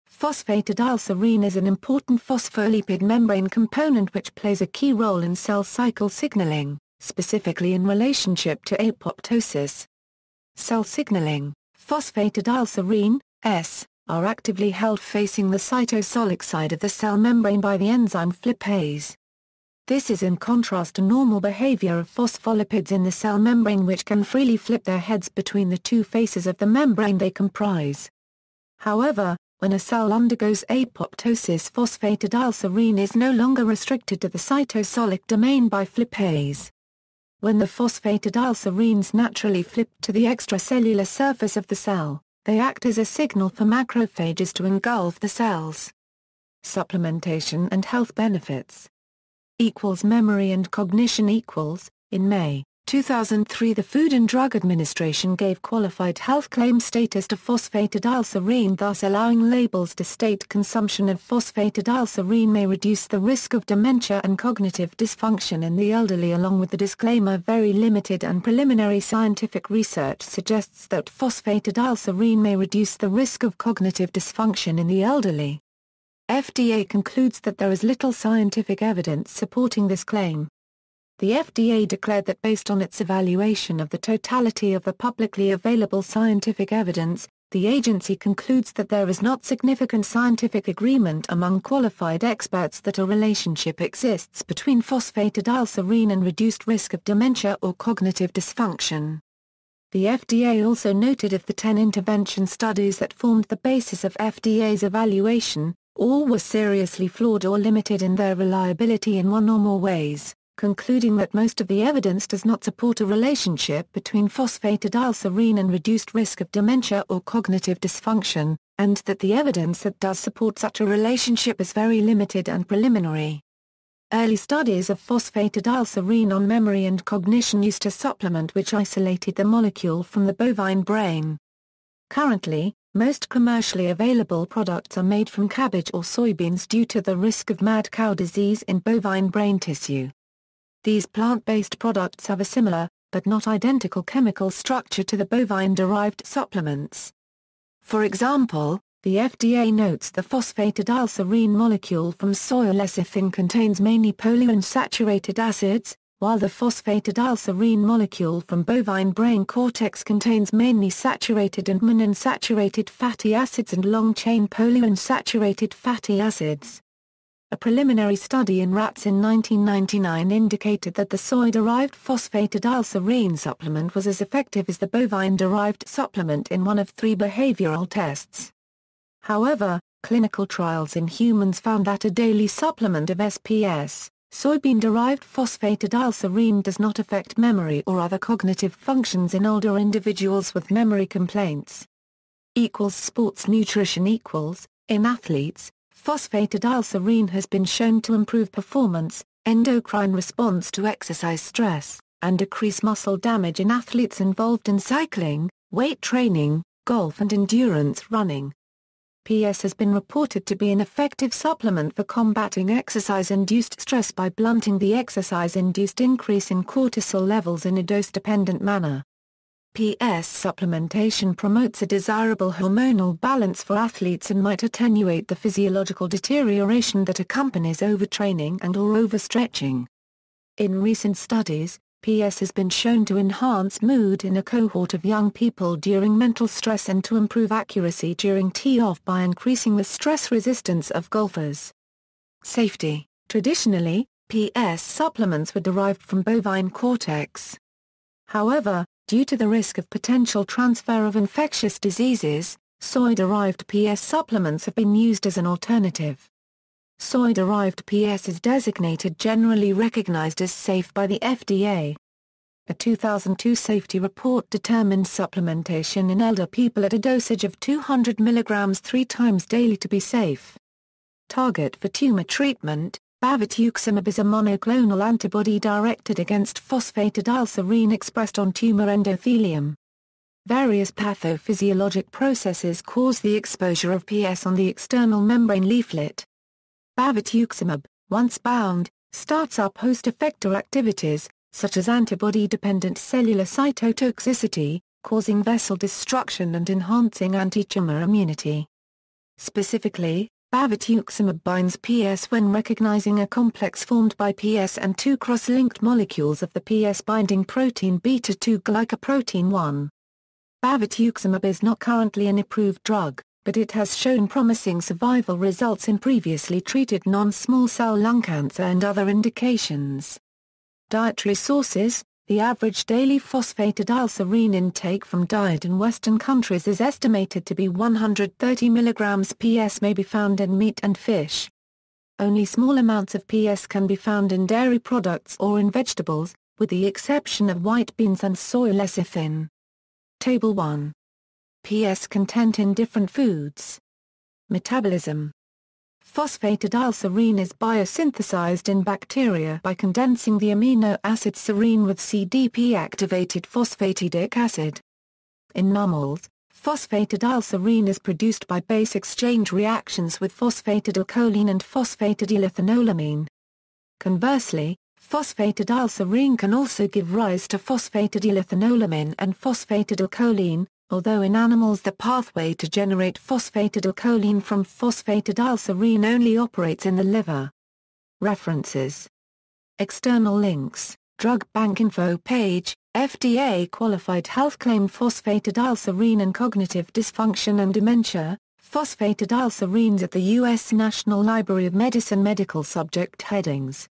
Phosphatidylserine
Phosphatidylserine is an important phospholipid membrane component which plays a key role in cell cycle signaling, specifically in relationship to apoptosis.